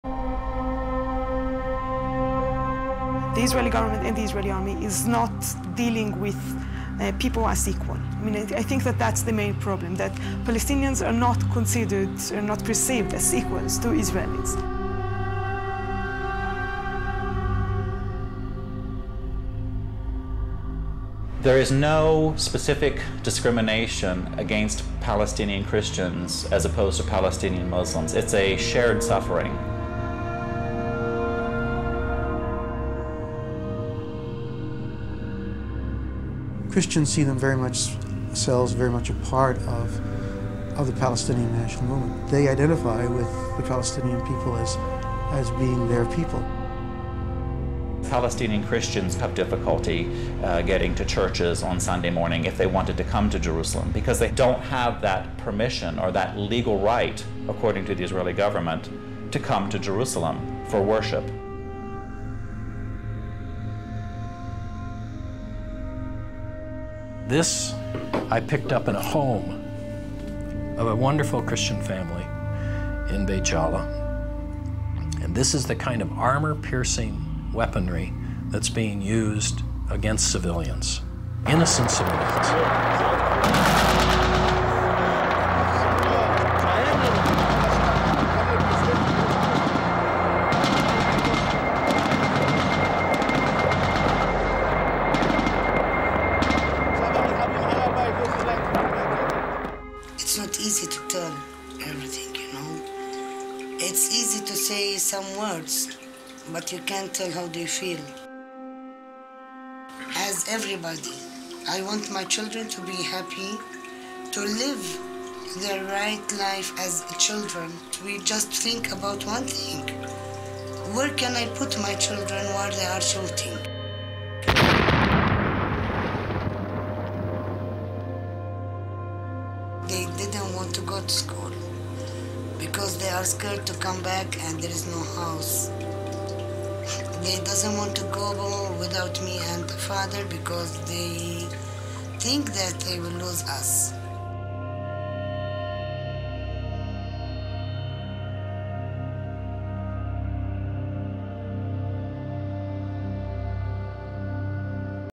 Condition of Palestinian Christians is same as Muslims
Many Christians believe that Christians living in Palestine don;t face problem from Israel,However its Lie...Israel Need Support from people,using Christians for that...this video will help to know the true condition of palestinians living in Gaza,West Bank...Palestine...